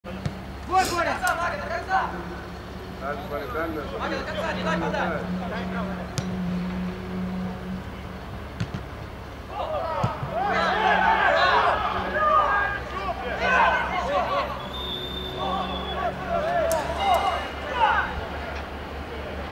А был ли гол? 2-1
2-1. "Сокол" Саратов - "Динамо" Вологда 2:1 (1:0). Контрольный матч. Адлер. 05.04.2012. .